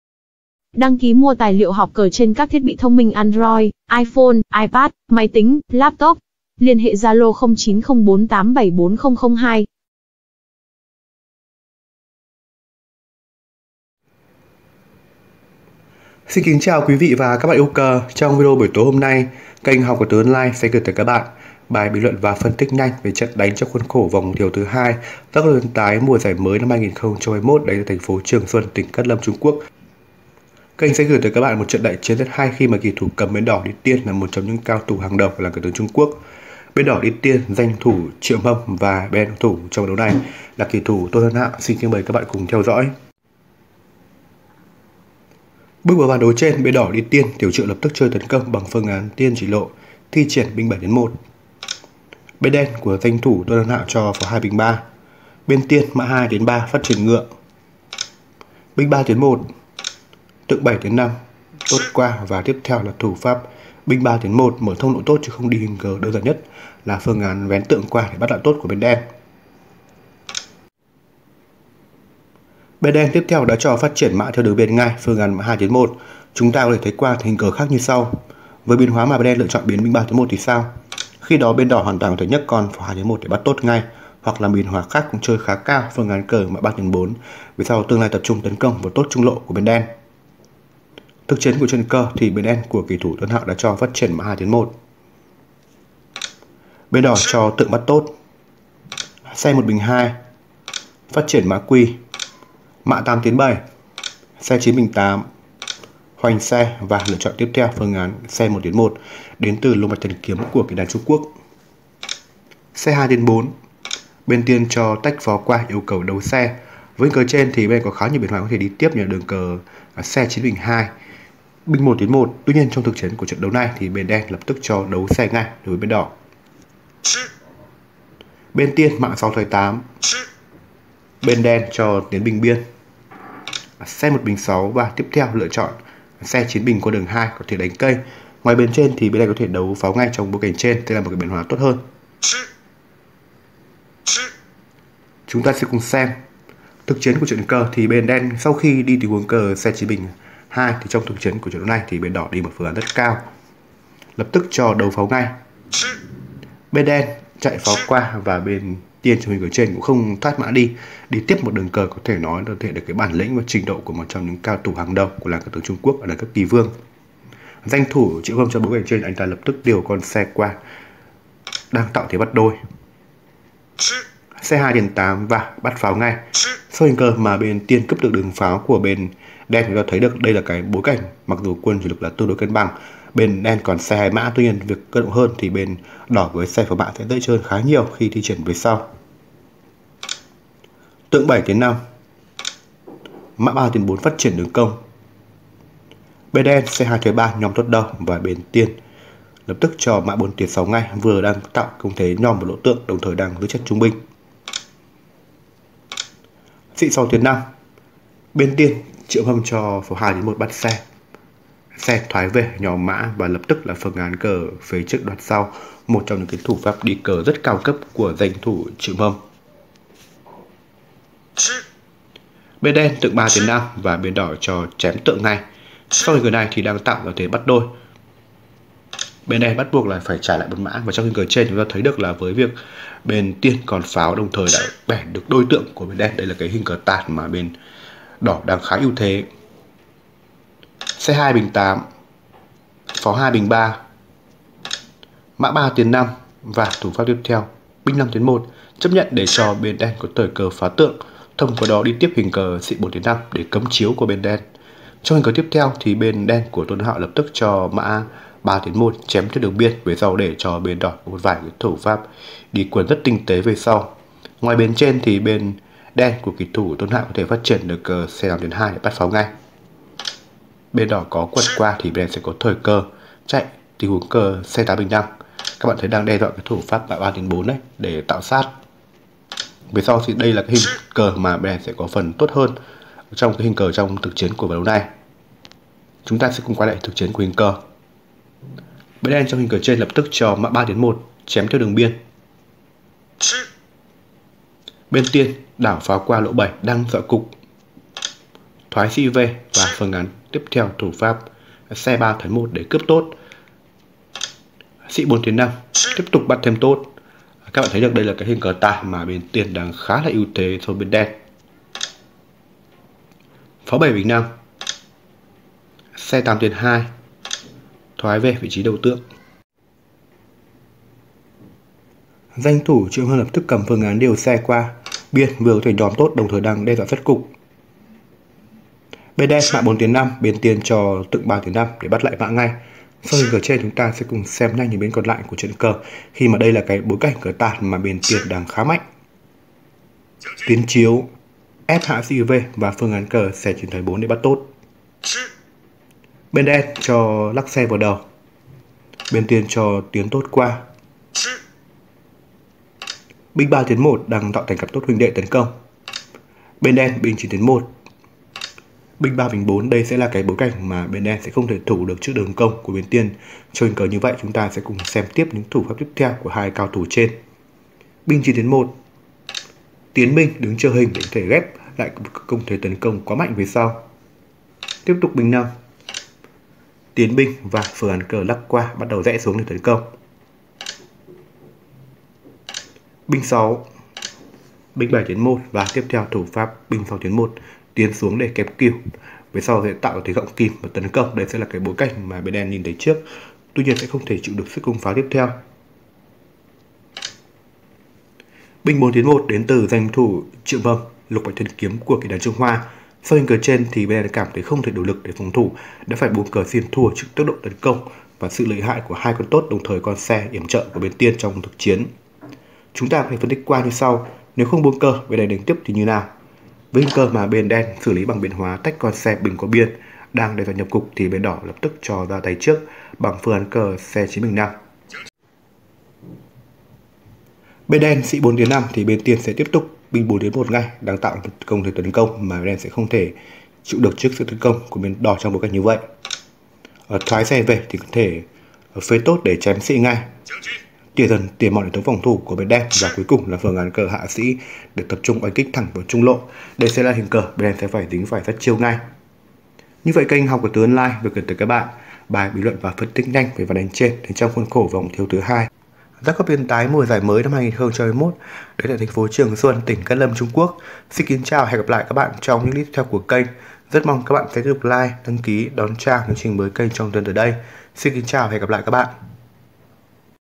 Tấn công đỉnh cao. Nghệ thuật vận quân siêu hạng của lục mạch thần kiếm Triệu Hâm Hâm
Bình luận cờ tướng đỉnh cao. Vòng 2 giáp cấp liên tái 2021. Triệu Hâm Hâm vs Tôn Hân Hạo.

Đăng ký mua tài liệu học cờ trên các thiết bị thông minh Android, Iphone, ipad, máy tính laptop.